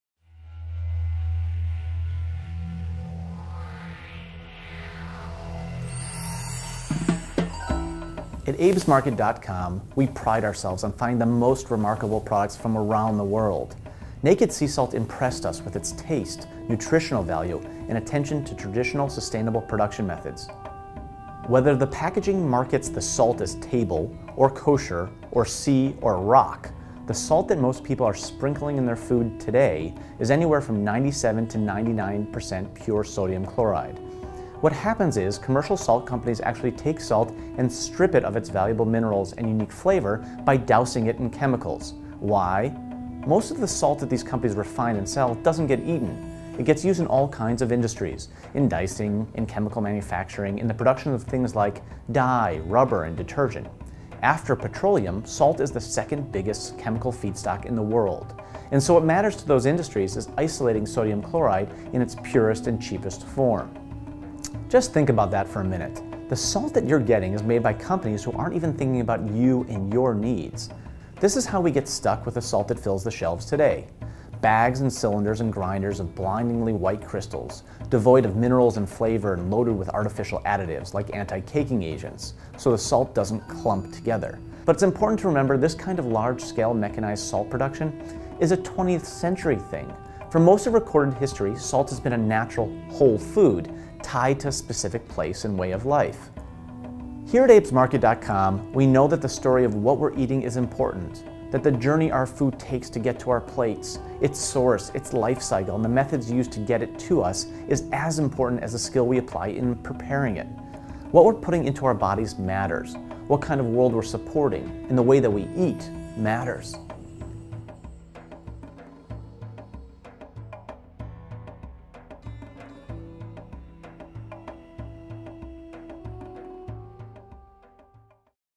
Naked Sea Salt - ABE'S MARKET Partnership
Naked Sea Salt's mission is to introduce the world to gourmet salt from the Dead Sea, an all-natural, low-sodium, and mineral-rich alternative to artificially refined salt. Founded by environmental activist Ari Leon Fruchter, the company encourages regional cooperation, working with a Palestinian company to harvest the salt from the Dead Sea with traditional, chemical-free methods that preserve the sea's unique mineral content. Each of Naked Sea Salt's sixteen flavors have been blended with organic herbs and spices to create a salt of bold, creative taste as well as unparalleled nutrition. With its dedication to environmental and economic concerns, part of the company's mission is to serve as a model of sustainable, ethical practice for private manufacturing companies in the region.

NakedSea Salt donates 3% of its Net Sales to the Arava Institute for Environmental Studies.